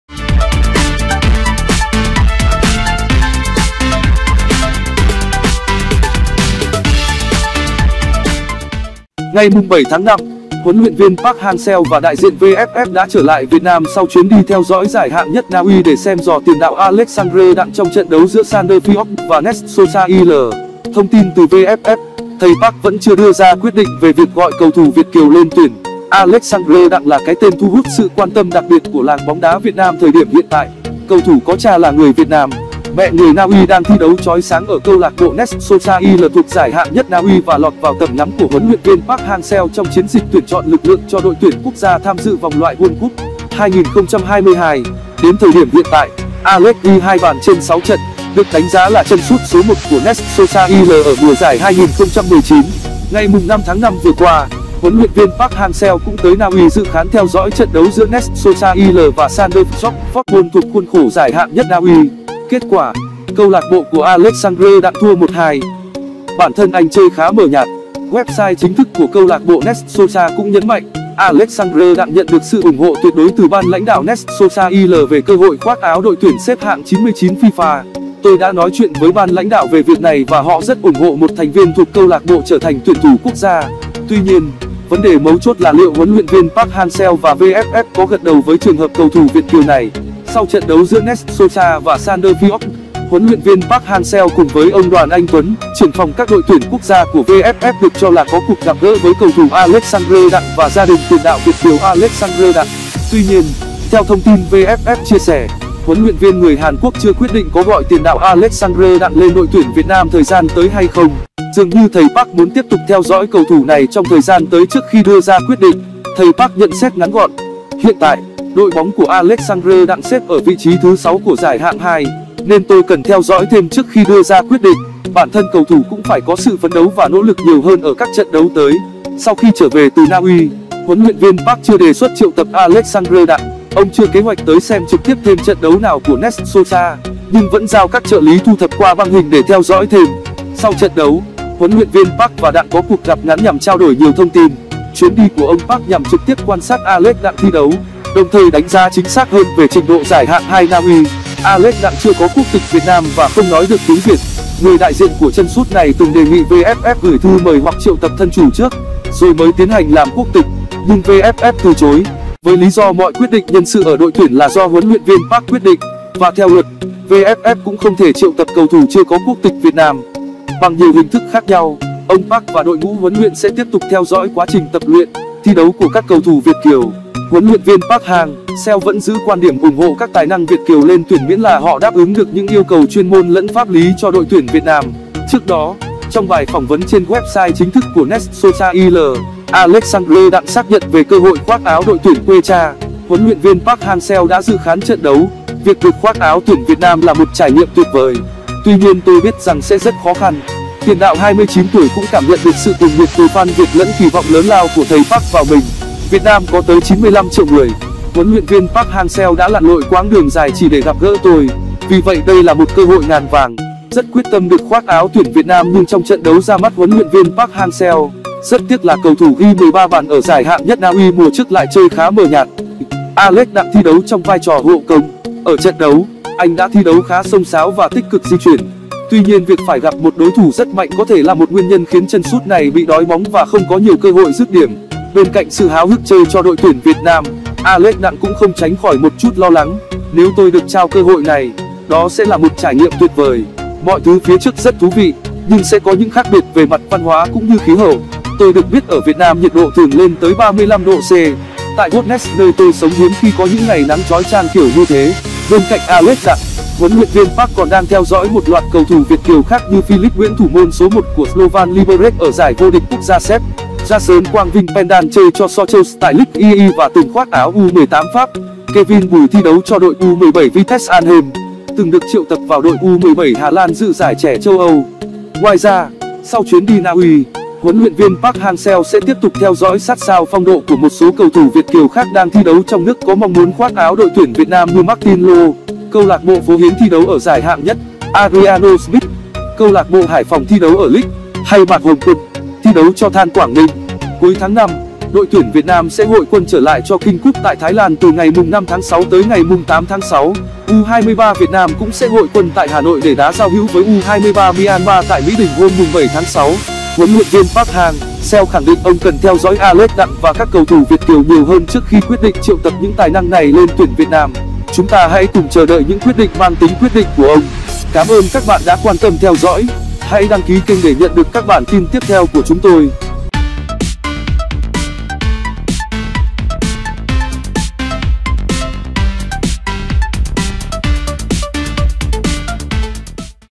Alexander Đặng không được HLV Park Hang Seo gọi vào ĐTVN
Alexander Đặng không được HLV Park Hang Seo gọi vào ĐTVN. Sau khi trở về từ Na Uy, HLV Park Hang-seo chưa đề xuất triệu tập Alexander Đặng. Ông không có kế hoạch tới xem trực tiếp thêm trận đấu nào của CLB Nest-Sotra IL, nhưng vẫn giao các trợ lý thu thập qua băng hình để theo dõi thêm.